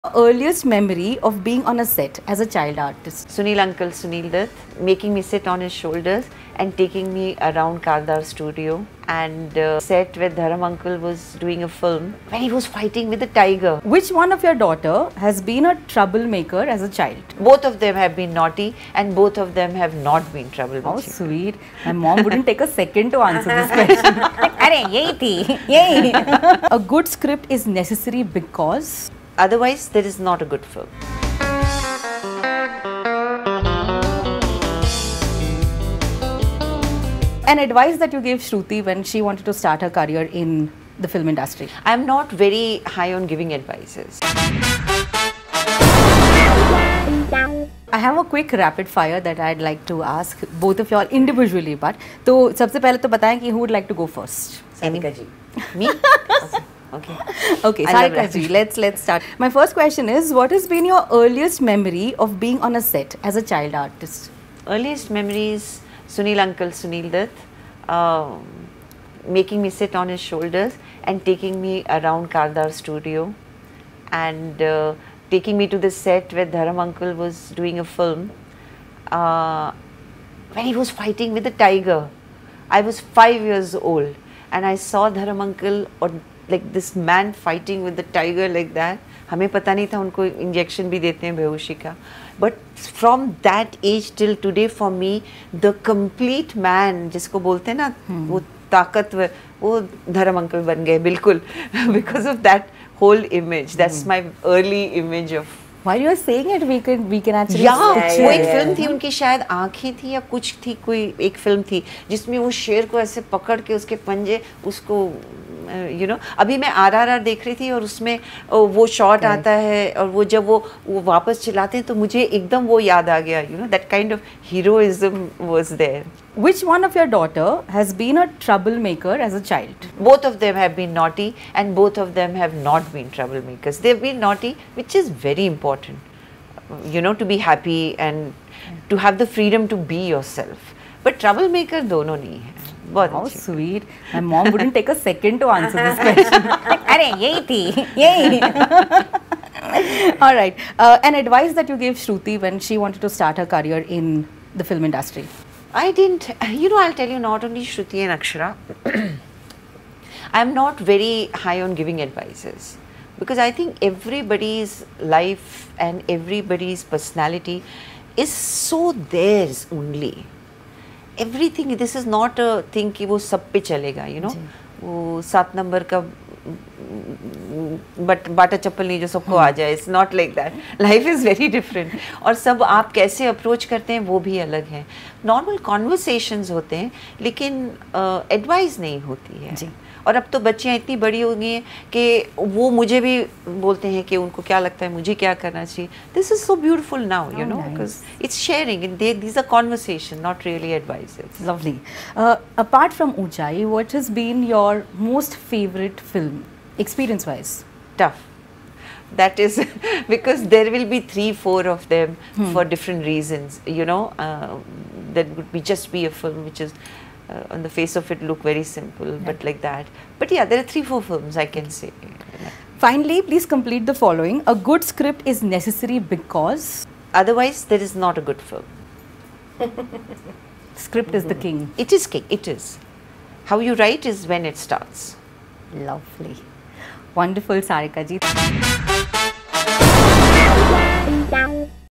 Sarika on advising Shruti Haasan: "Advice 7 no. ka Bata chappal nahin jo sab pe chalega"| Rapid Fire
Sarika in an exclusive interview with Bollywood Hungama's Urmin Vijayakar aka RJ Urmin replies on heartwarming rapid fire questions on JrNTR, RRR, SunilDutt, Dharmendra, ShrutiHaasan, and much more. Don't miss!